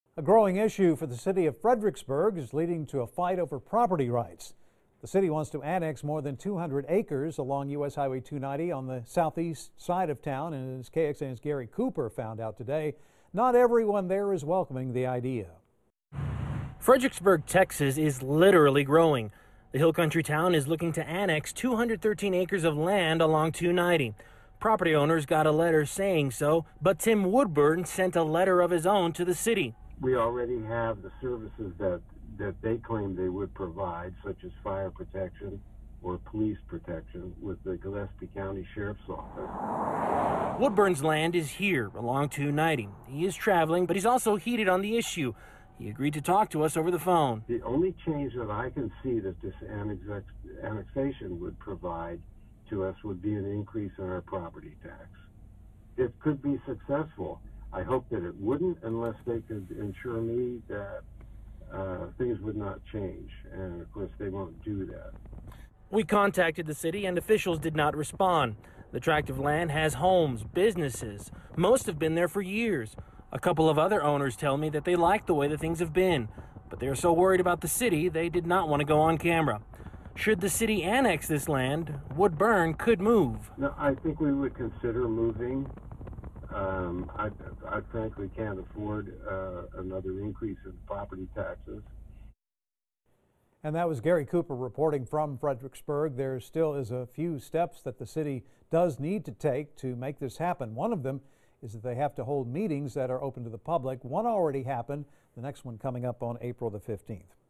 Fredericksburg has a growing issue about its growth
KXAN's Gary Cooper found out not everyone is welcoming the idea.